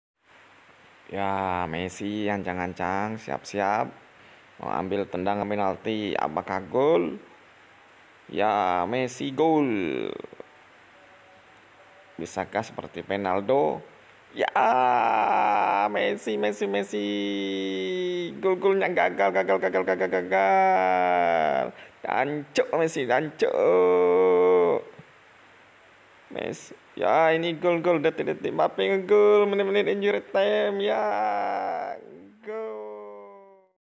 Skor leg 1 UCL PSG vs Madrid || Penalty Messi vs Goll injury time Mbape
Hasil leg 1 UCL PSG vs Madrid
Hasil dimenangkan PSG 1:0
Messi gagal Penalty